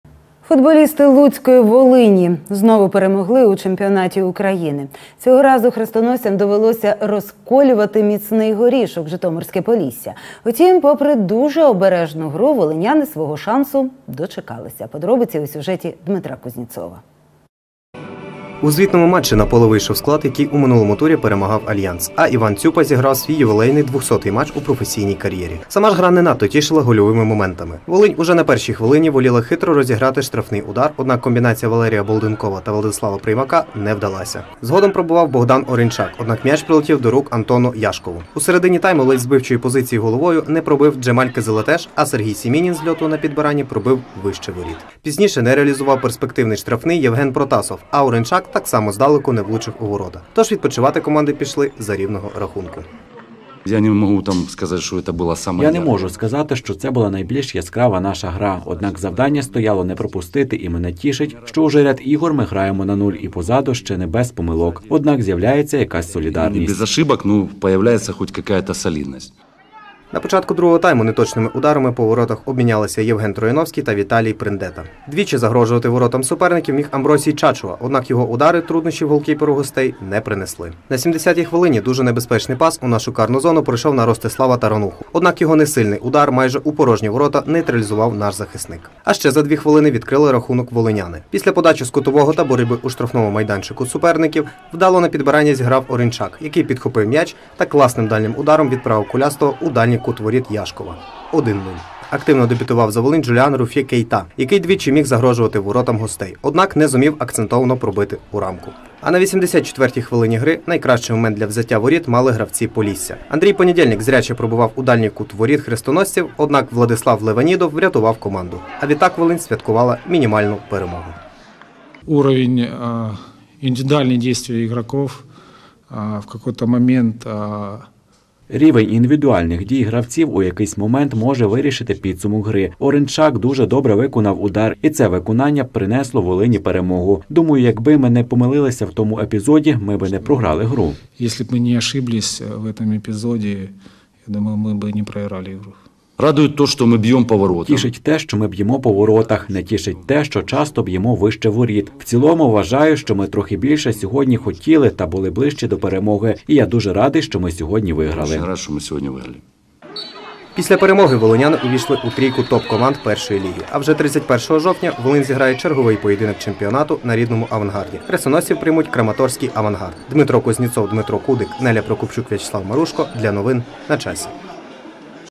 Ще одна перемога луцької «Волині» у чемпіонаті України
Футболісти луцької «Волині» знову перемогли у чемпіонаті України. Цього разу «Хрестоносцям» довелося розколювати міцний горішок – житомирське «Полісся». Утім, попри дуже обережну гру волиняни свого шансу дочекалися.